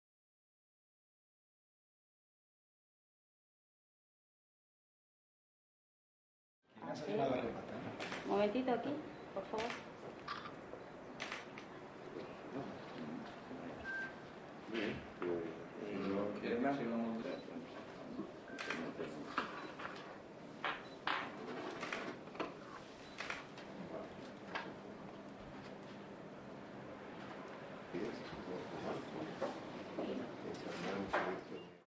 Firma de la Escritura de la Fundacion Andalucía Tech
Los rectores de Sevilla y Málaga firman la escritura de constitución de la Fundación Andalucía Tech